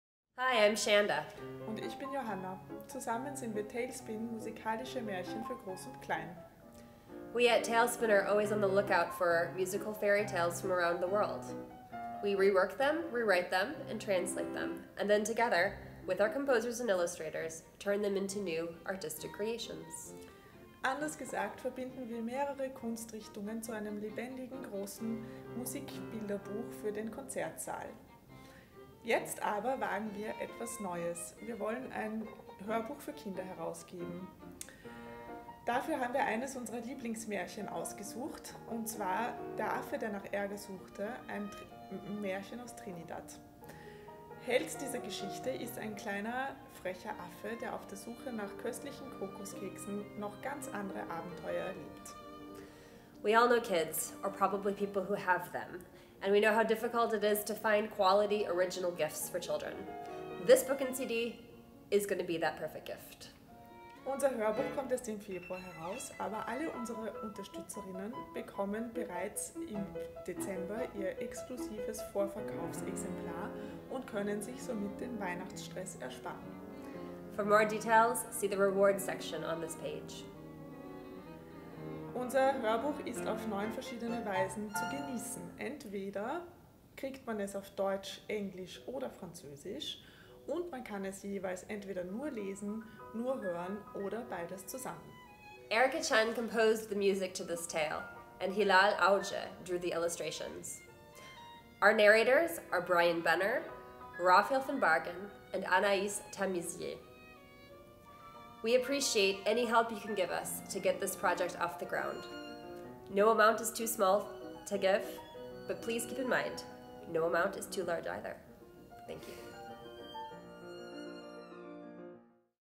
Talespin "Monkey" Book & CD Trailer
Talespin is putting out its first book for kids in three languages and we are here to tell you all about it!